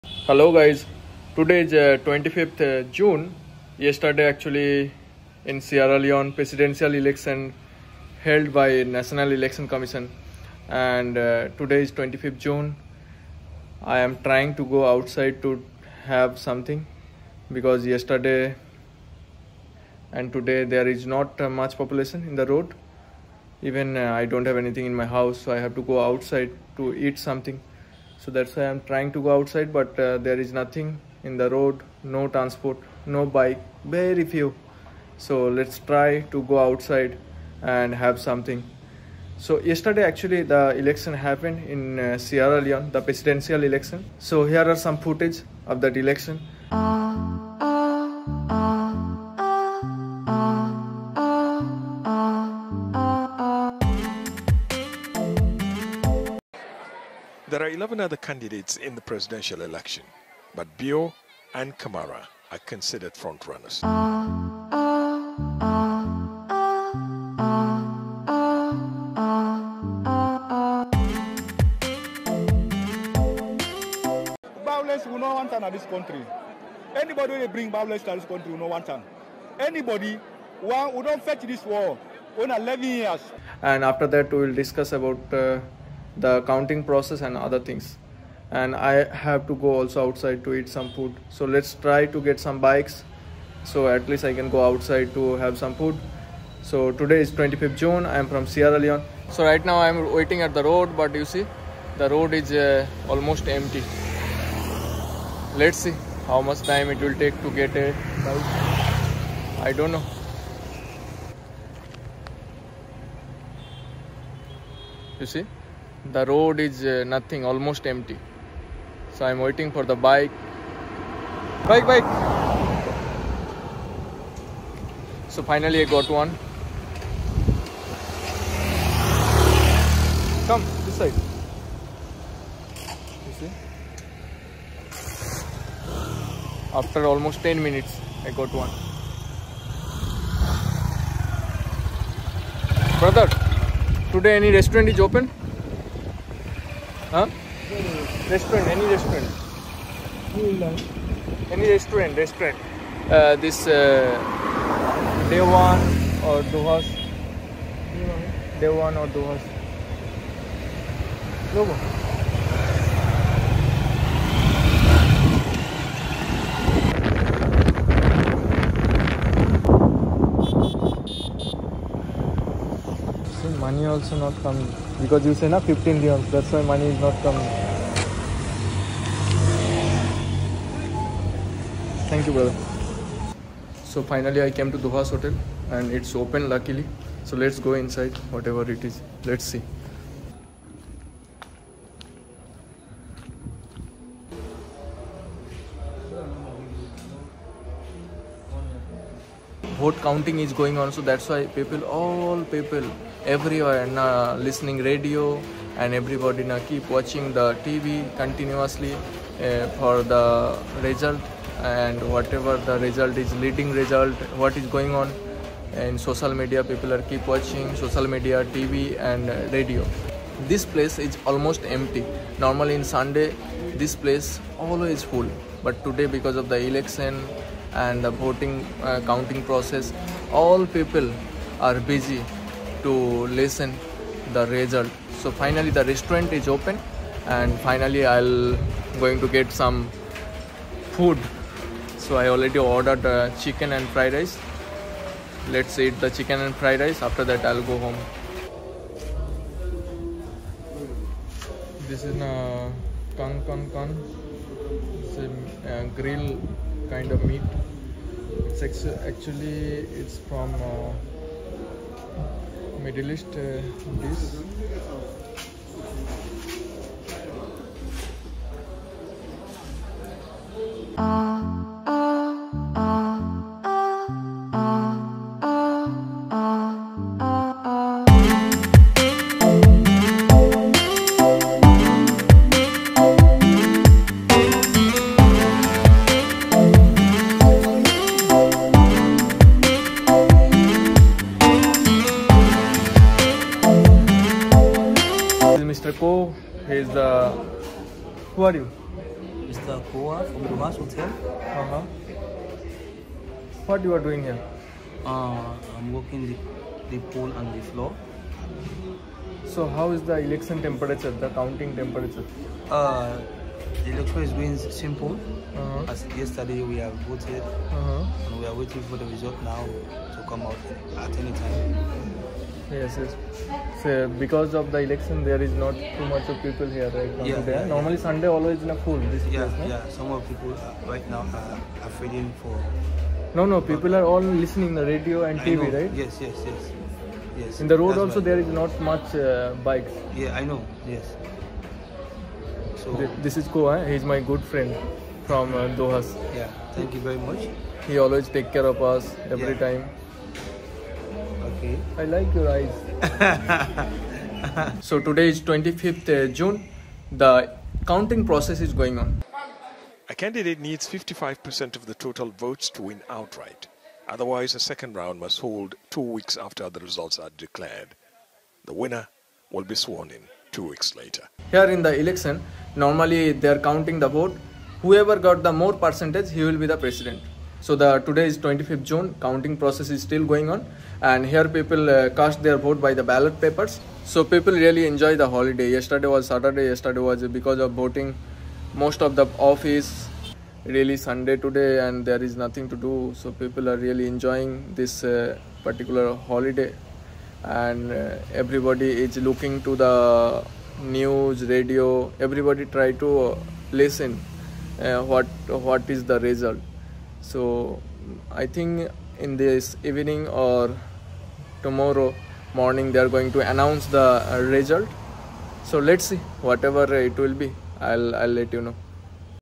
Sierra Leone Election 2023
The recently concluded Sierra Leone election held yesterday marked a significant milestone in the nation's democratic journey. It was a day of civic engagement, where Sierra Leoneans exercised their fundamental right to vote and actively participated in shaping the future of their beloved country.

The election witnessed a commendable display of political maturity and peaceful democratic processes, reflecting the growing strength of Sierra Leone's democratic institutions. Citizens turned out in large numbers, showcasing their commitment to the democratic process and their desire to contribute to a better Sierra Leone.